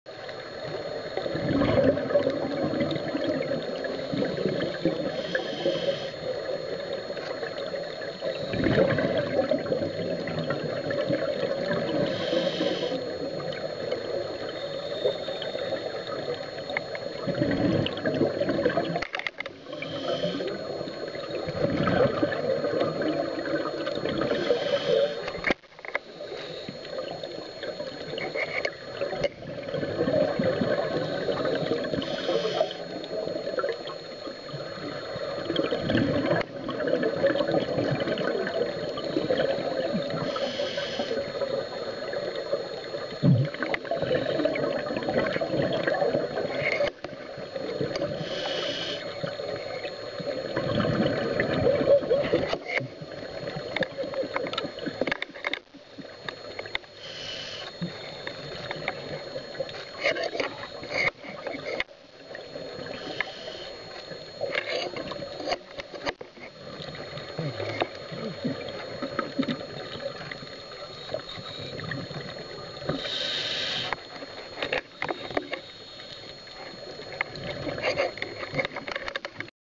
Playing with a Sea Cucumber
Filmed it in Hurgada, Abu Haschish South, on 1st of januari 2008